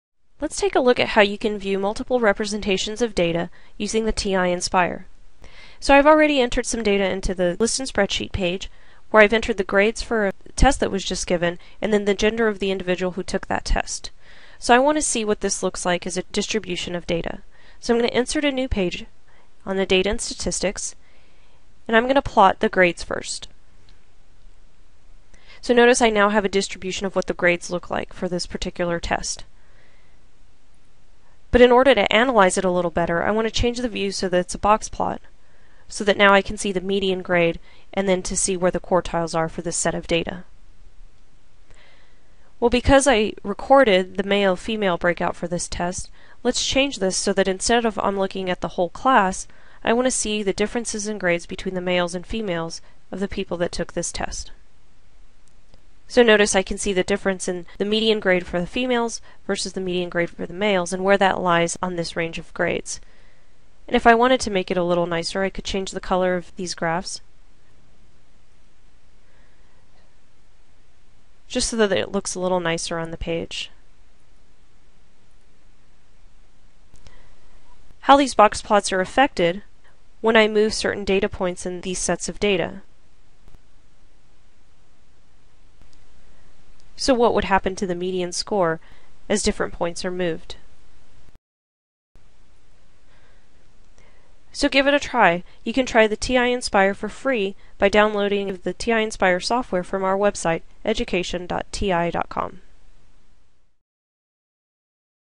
Criar diagramas de caixa para dados na TI-Nspire CX
Este tutorial ensina a comparar dois tipos de dados utilizando diagramas de caixa na calculadora TI-Nspire CX. A tecnologia TI-Nspire CX permite uma compreensão mais aprofundada de conceitos abstratos nas disciplinas de Matemática e Ciências. Esta calculadora é autorizada nos Exames Nacionais de Matemática do Ensino Secundário.